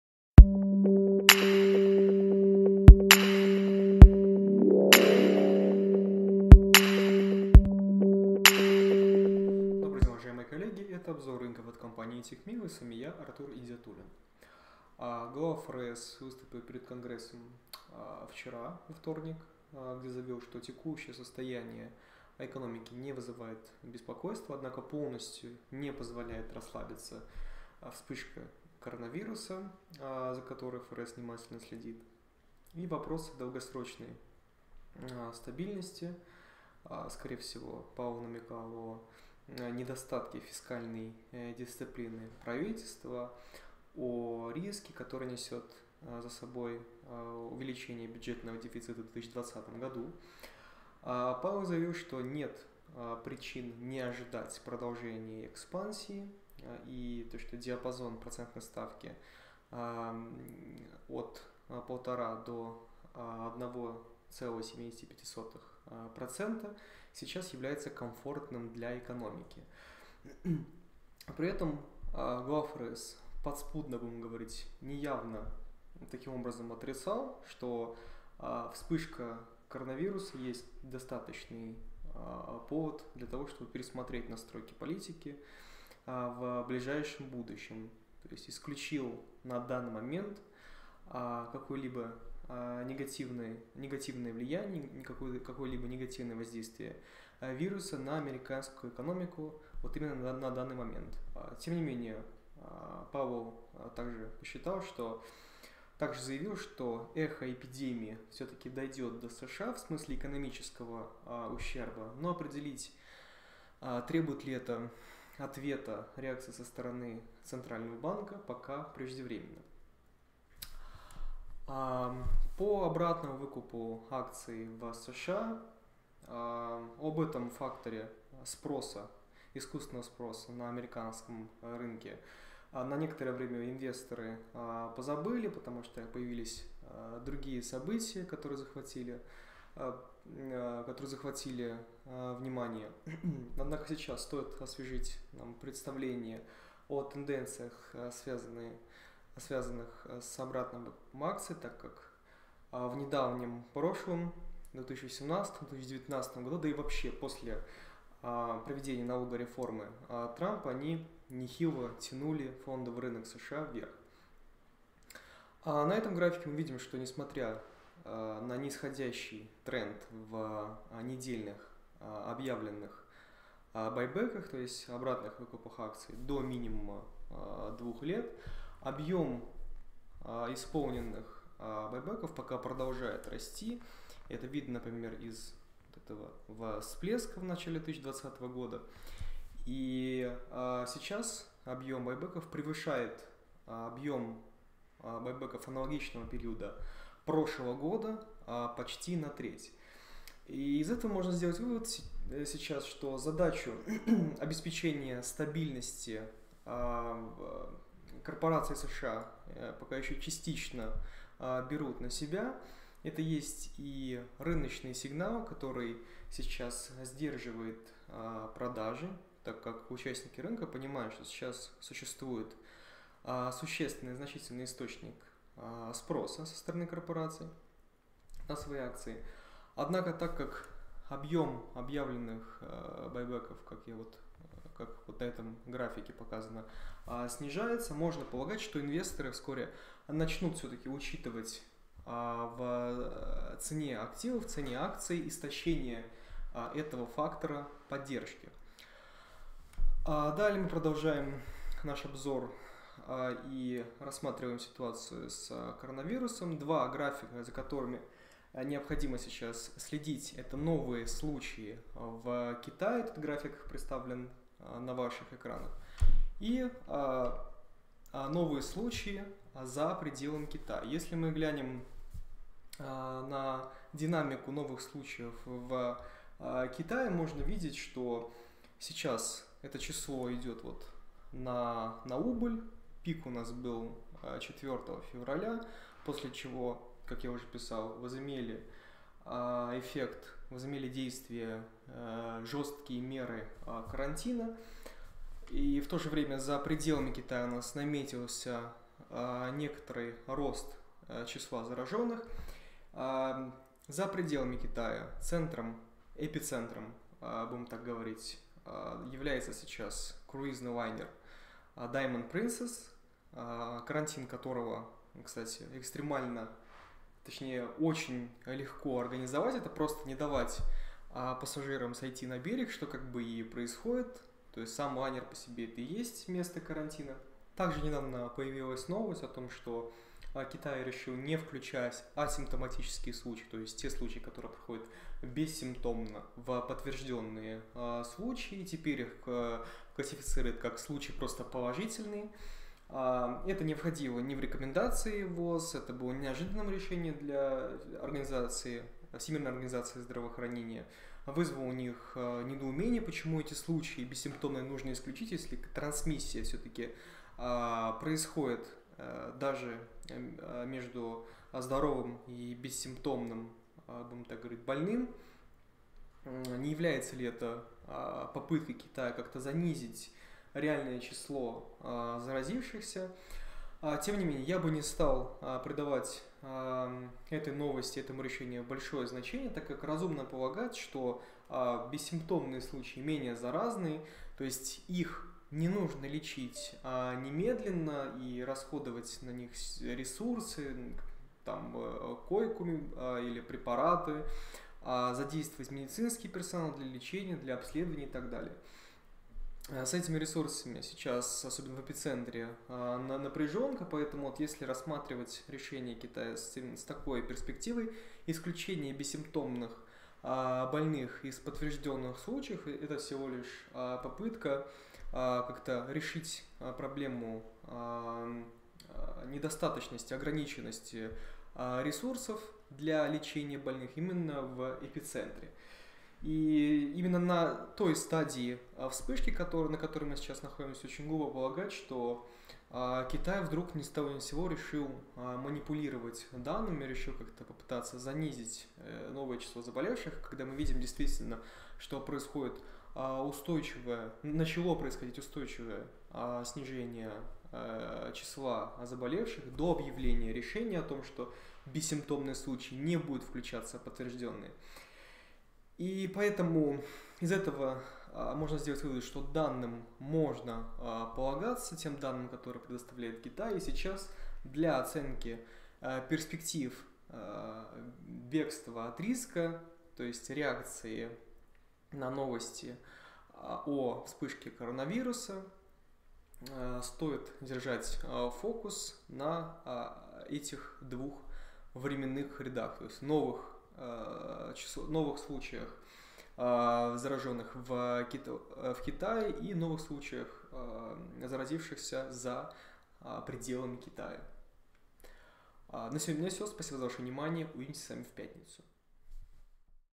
Пауэлл, обратный выкуп акций и коронавирус – обзор рынка 12.02. 2020
- Глава ФРС Пауэлл: Продолжаем наращивать активы на балансе

- Обратный выкуп акций: +27% в начале года по сравнению с тем же периодом в прошлом году.

- Два графика про коронавирус за которыми нужно следить